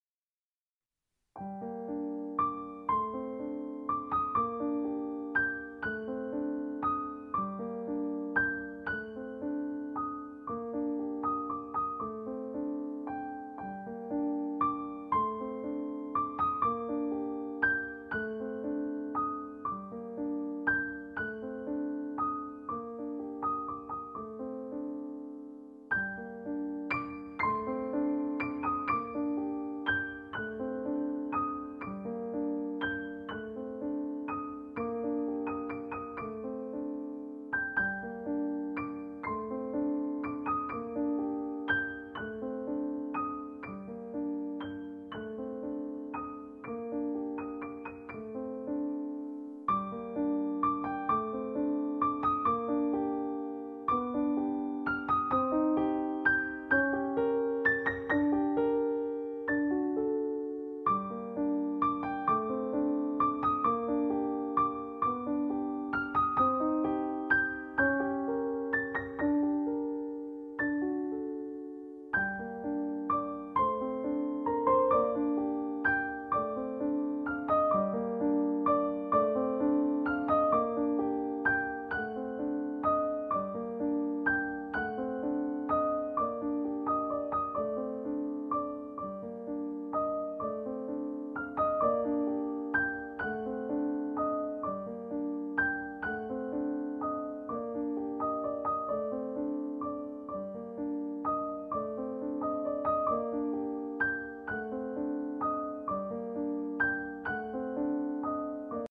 Homeless cat cat catlover catvideos cutecat homeless streetcats stray
Homeless cat hungry in the morning